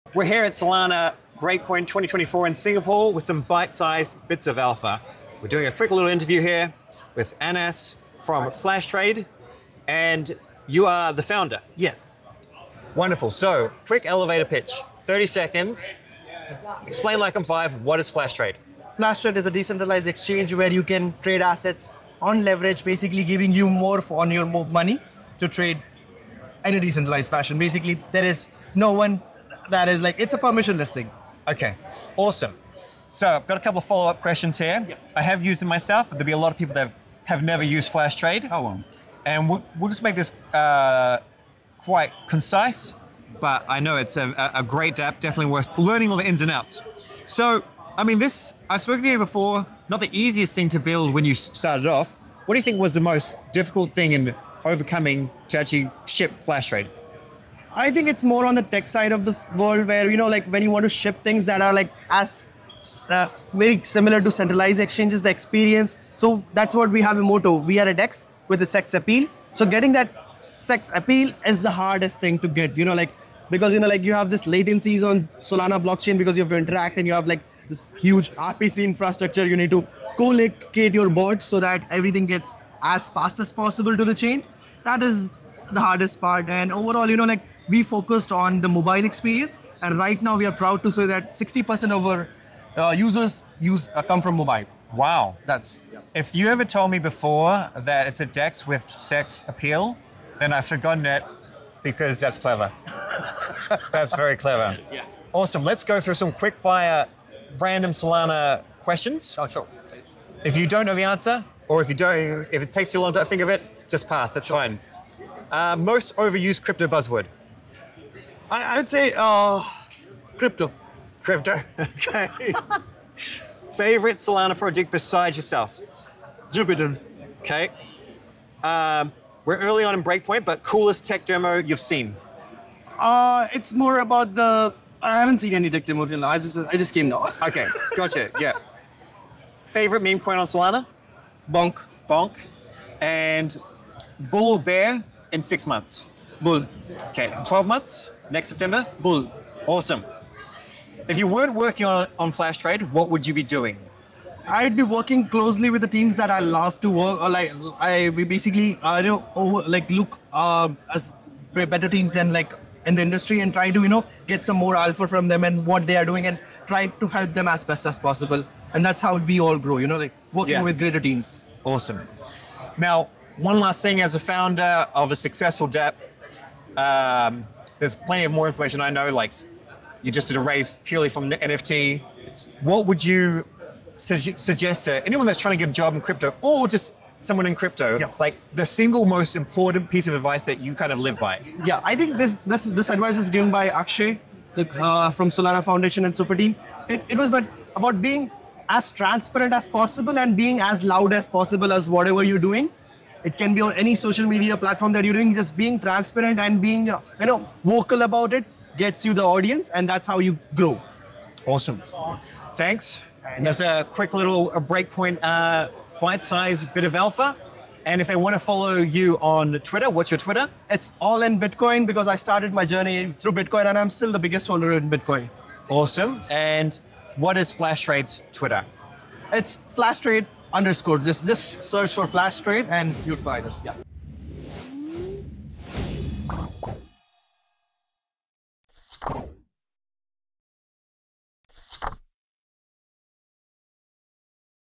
Solana Breakpoint 2024: Quick Alpha Dump on Flash Trade with Anas
In this video we interview Anas from Flash Trade at Solana Breakpoint Singapore 2024.

Follow Flash Trade on X!

Who Do I Stake My SOL With?

⏰Timestamps⏰
00:00 Introduction to Solana Breakpoint 2024
00:05 Meet Anas from FlashTrade
00:16 What is FlashTrade?
00:47 Challenges in Building FlashTrade
01:26 Mobile Experience and User Base
01:45 Quickfire Solana Questions
02:52 Advice for Aspiring Crypto Professionals
03:33 Closing Remarks and Social Media

What Wallet?

Solana 👉 2coZyEDezNyzTvfAYkAJRZZGabsx4JtA8XFzXTLCCRN7

I am not a financial advisor. This is an educational tutorial and guide.